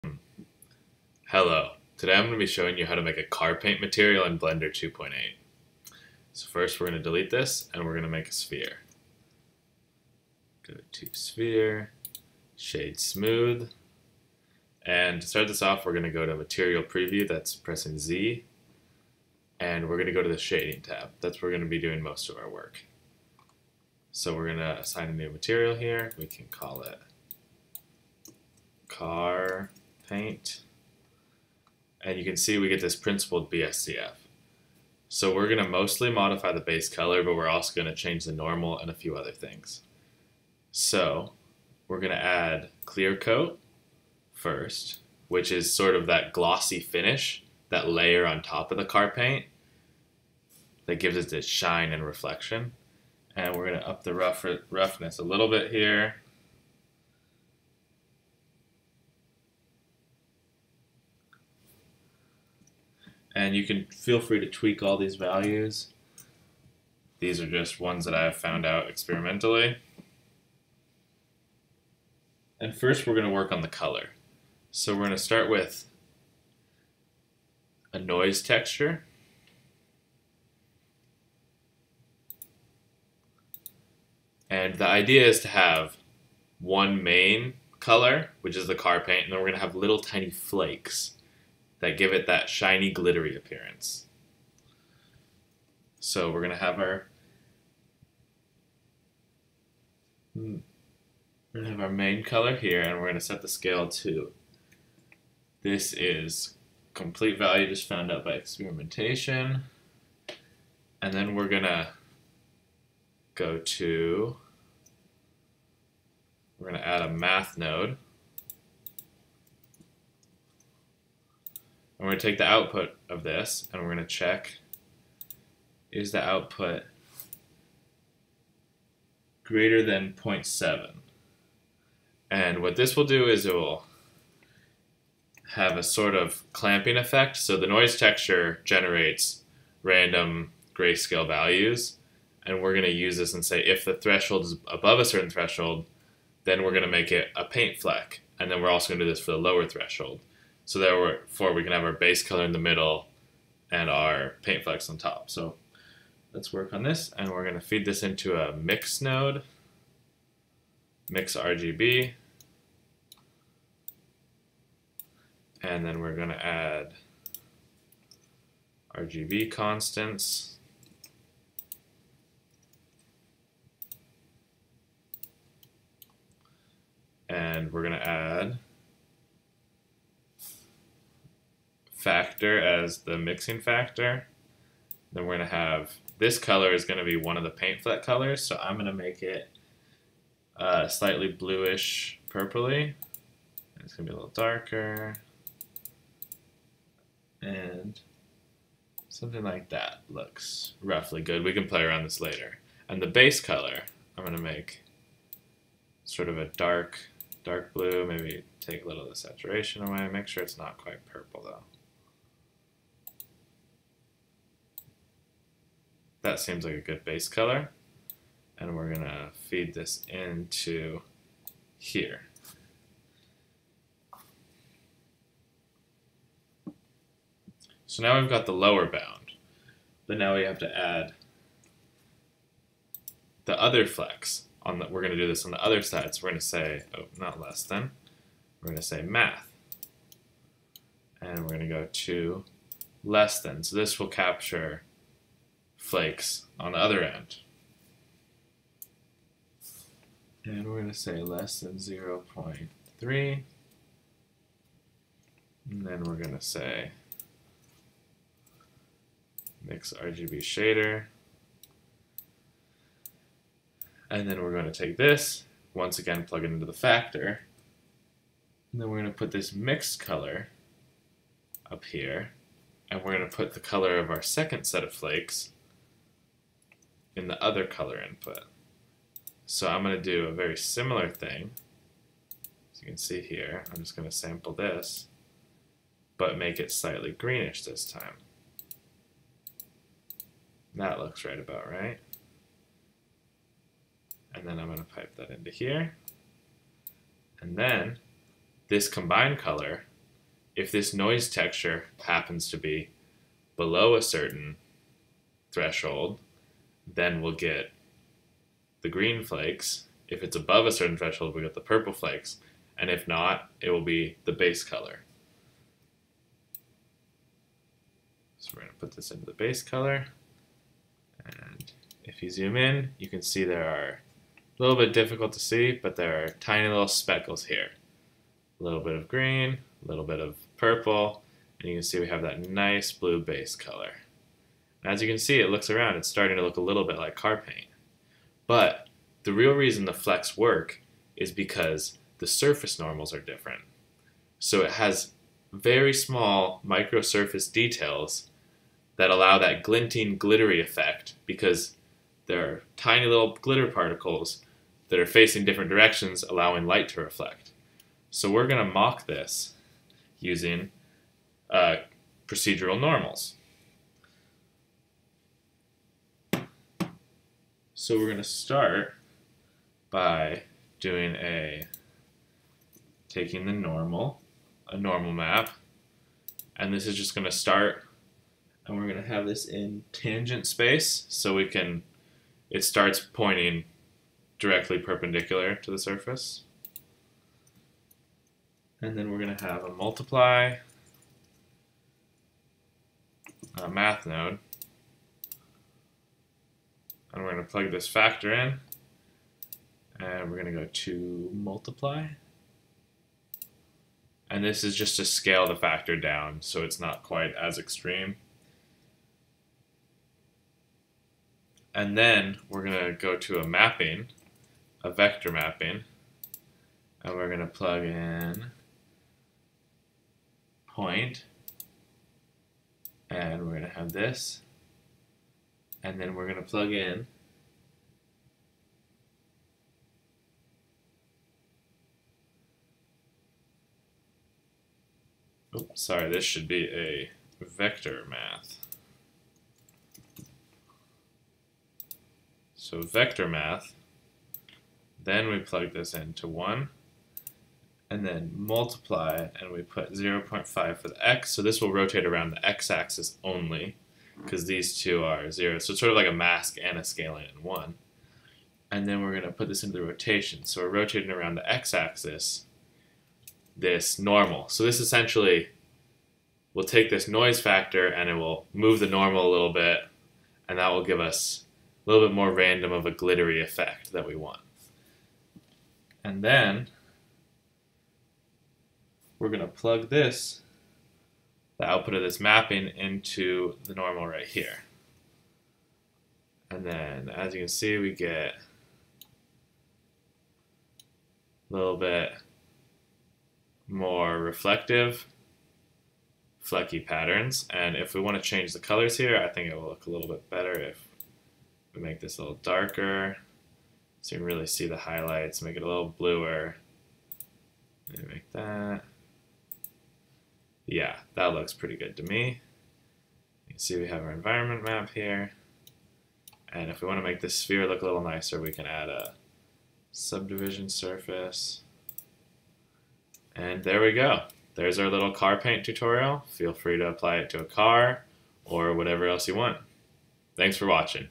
Creating a Car Paint Material - Blender 2.8
This is a quick tutorial on how to create a simple material in blender that looks like car paint.